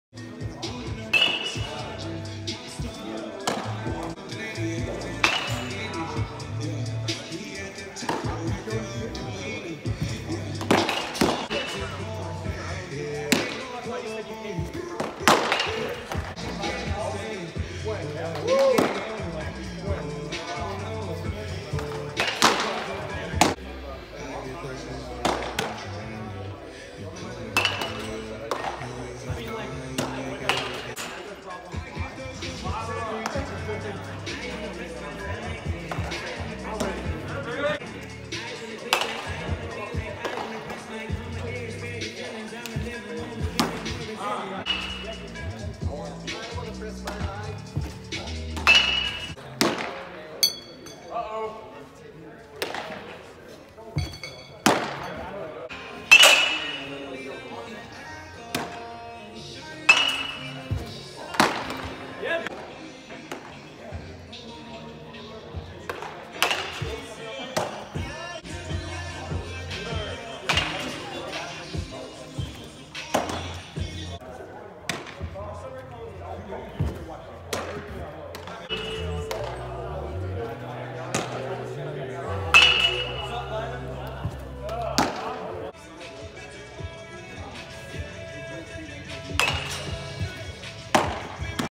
Andrew Altstadt- Spring Bullpen 3/12/19
Sophomore RHP at Northeast Community College in Norfolk, Nebraska.